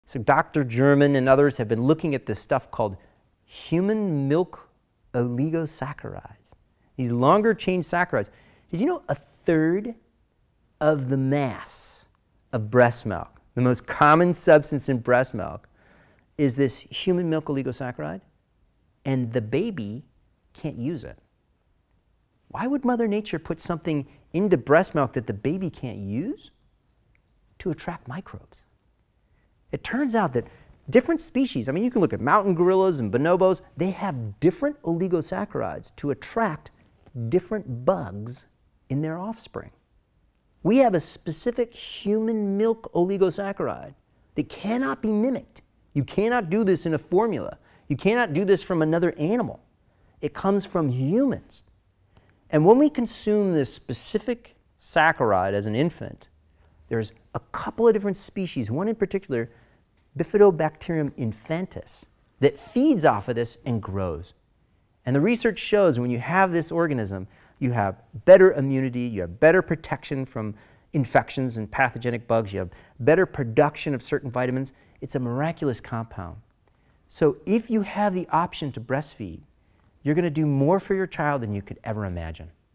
How breastfeeding builds a healthy microbiome
HMOs- Human Milk Oligosaccharides feed very specific microbes in the human infant allowing for a stronger immune system, better production of vitamins, and protect the infant from harmful infections.